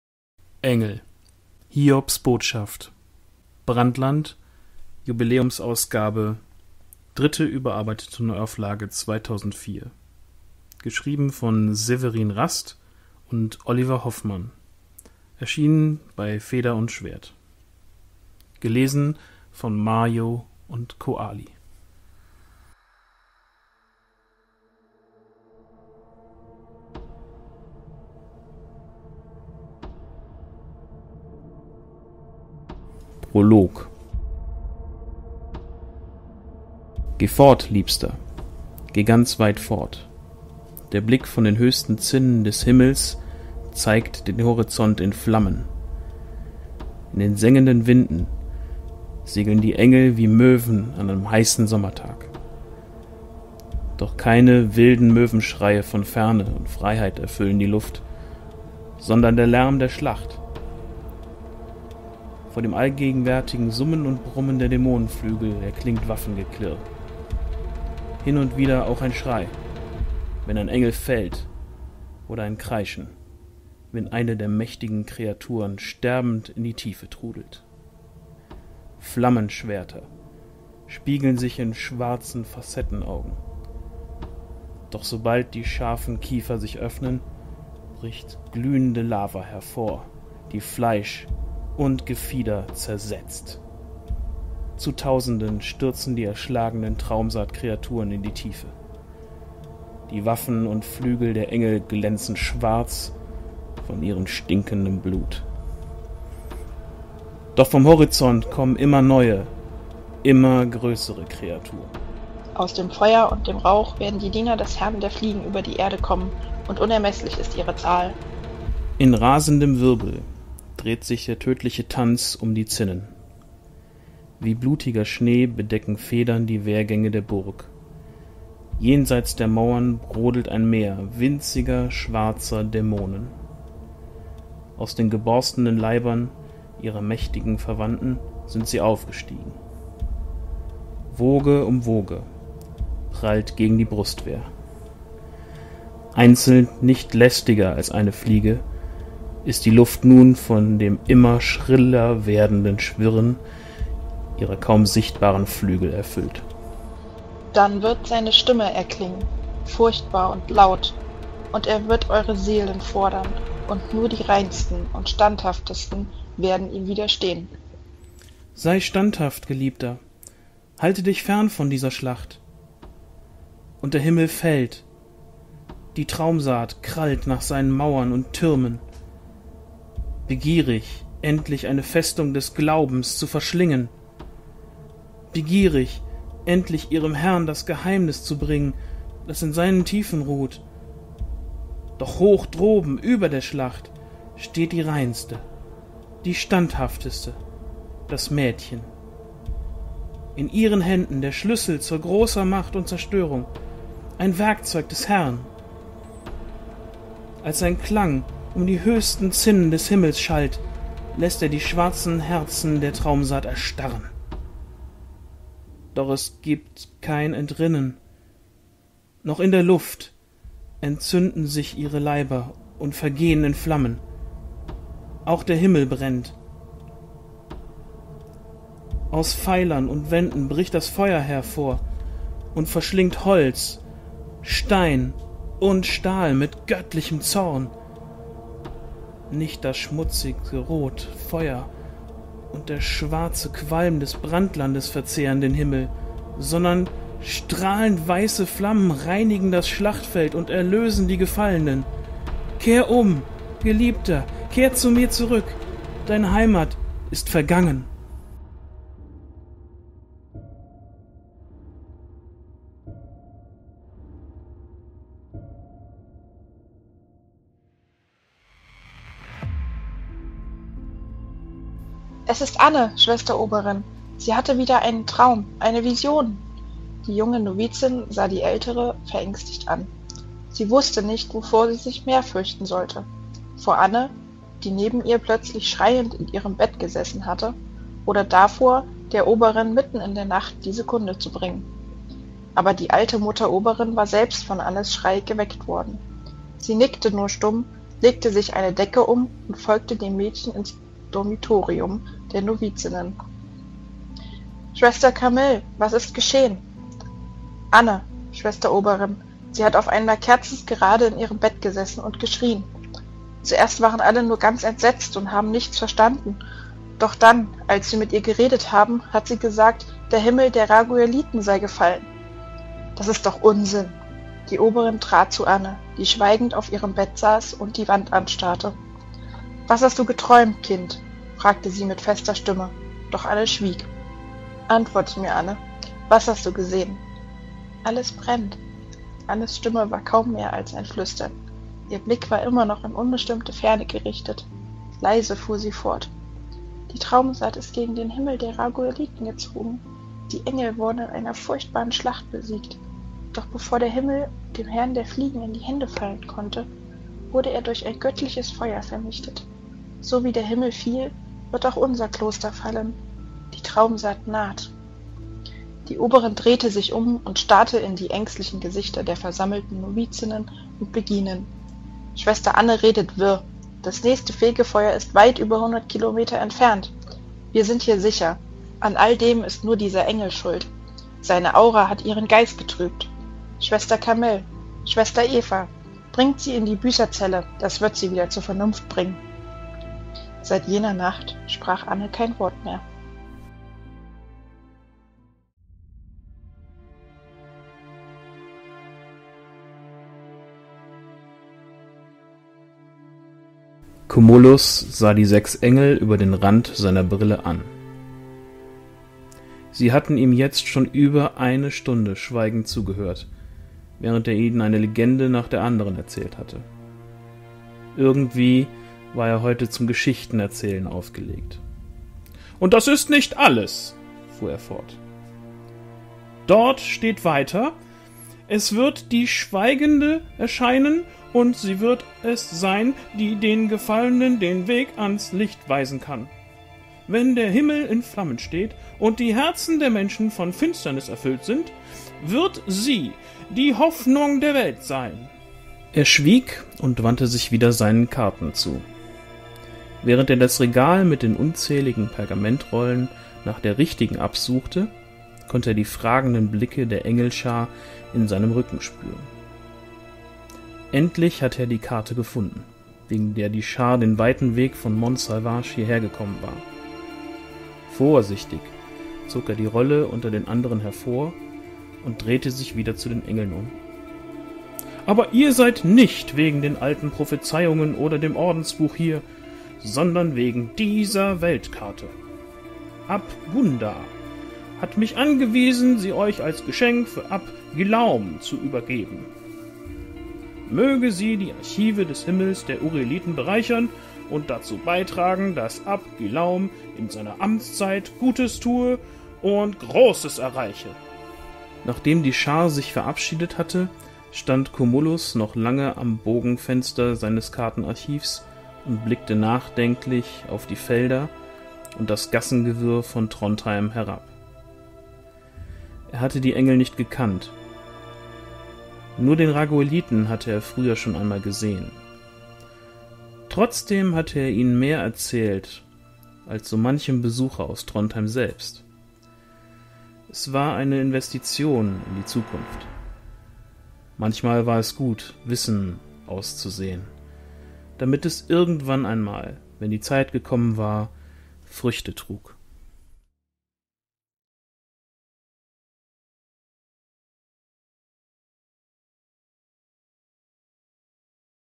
[Lesung] Engel: Hiobs Botschaft - Brandland - Prolog
Lesung von:

Engel: Hiobs Botschaft – Brandland

von Severin Rast und Oliver Hoffmann

© Feder & Schwert 2002
3. überarbeitete Neuauflage 2004

gelesen von MarJo und Koali

mit freundlicher Genehmigung von Feder & Schwert sowie den Autoren

Infos zum Roman:
„Brandland ist der erste Roman zur Welt der Chroniken der Engel. In einer fernen Zukunft lebt die Menschheit wieder auf dem Stand des Mittelalters und die Engel der Kirche versuchen die Menschen vor der Vernichtung durch die finstere Traumsaat des Herrn der Fliegen zu beschützen.
Seit jeher sind Engel die Boten des Herrn, und so beginnt auch diese Geschichte mit dem Botenflug eines Engels. Allein bricht der Raguelit Calliel mit einer Botschaft für das Kloster Cluny vom sturmumtosten Himmel zu Mont Salvage auf, während in Frankreich der Krieg gegen die furchtbare Traumsaat tobt.
Doch vielleicht ist es nicht der verzweifelte Kampf der himmlischen Heerscharen gegen die dunkle Brut des Herrn der Fliegen, der über die Zukunft der Welt entscheidet, sondern der Wille eines Schöpfers, der zahlreiche Figuren - Engel wie Menschen - über ein Spielfeld bewegt, das ganz Europa umfaßt.“ (Quelle: amazon.de)

Mehr Infos zum Pen and Paper Rollenspiel Engel auf dem der Roman basiert: